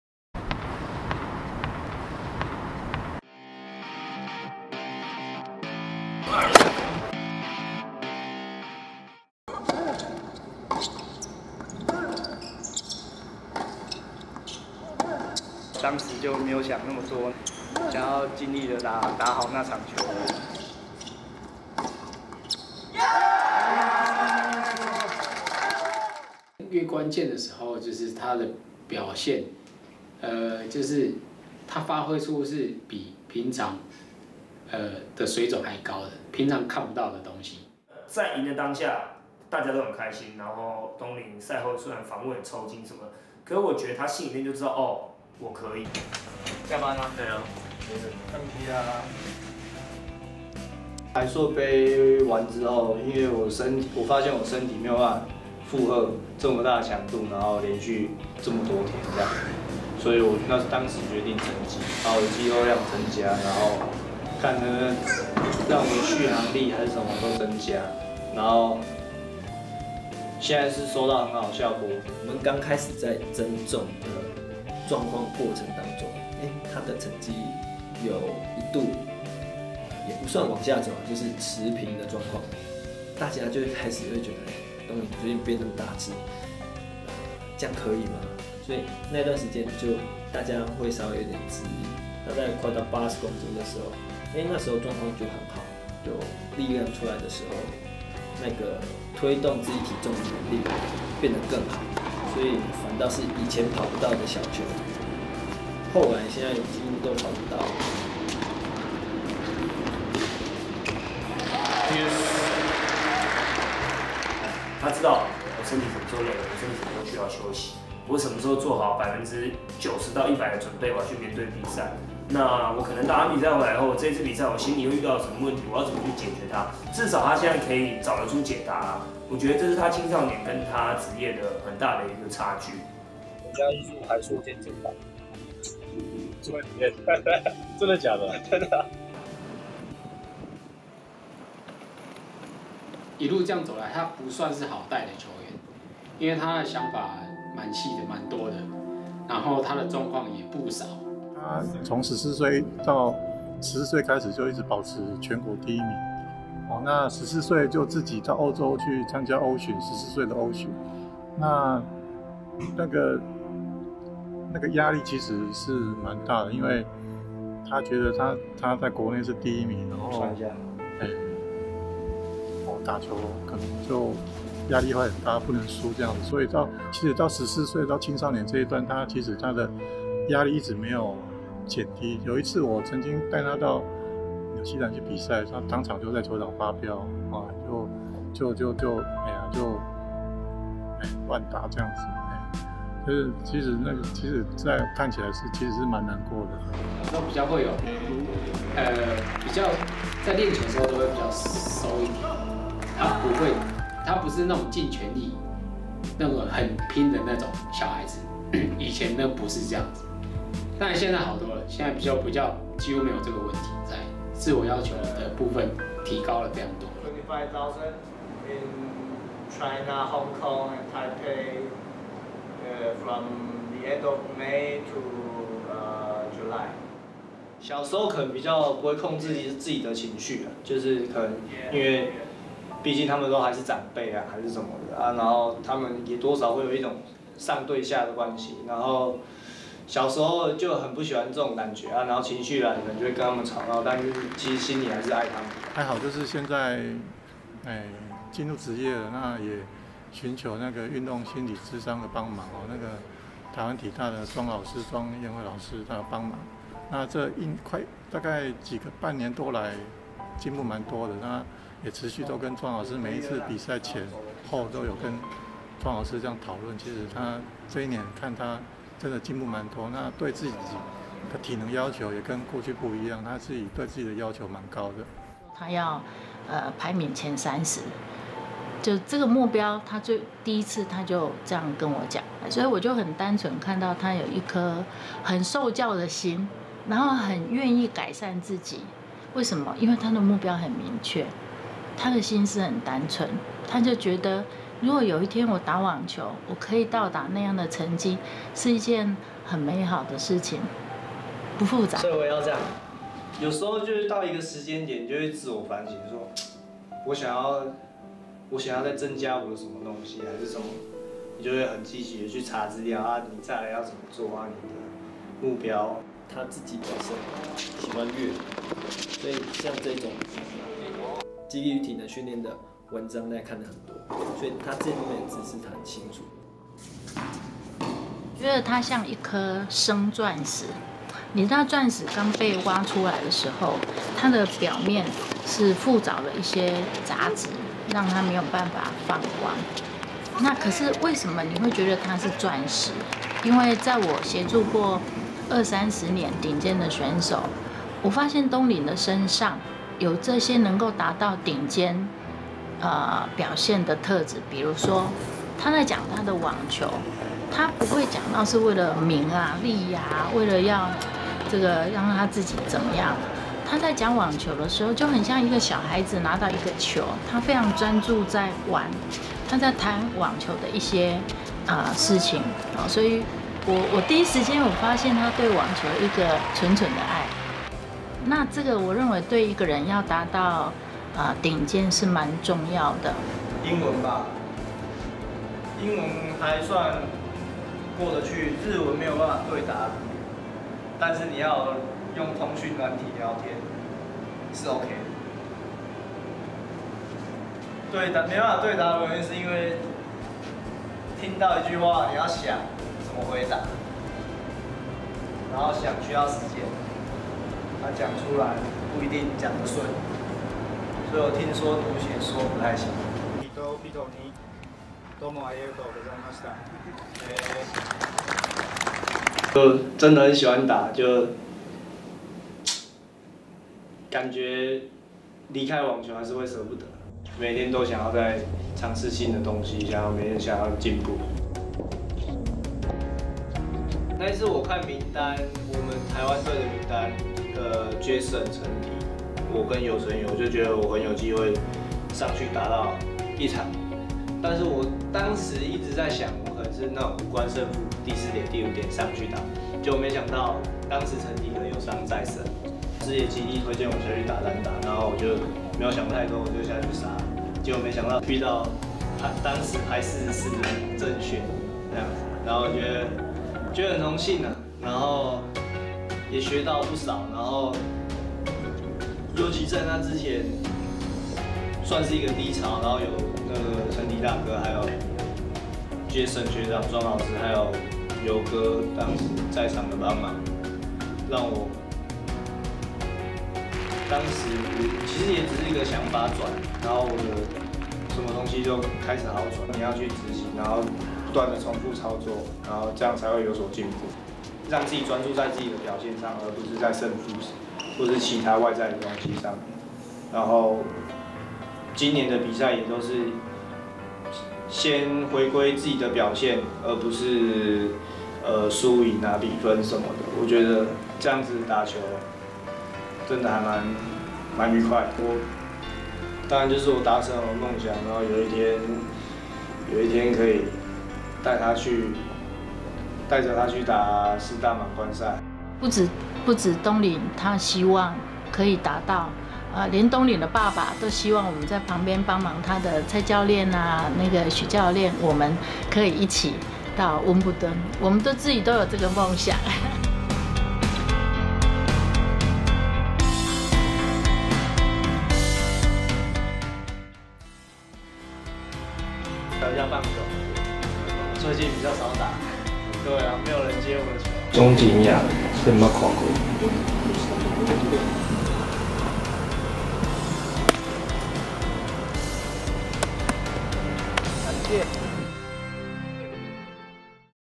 網球哲學實習生-吳東霖
台灣網球新生代選手吳東霖，對網球有著最單純的熱情，在挑戰職業網球的路上，一步一步往目標邁進。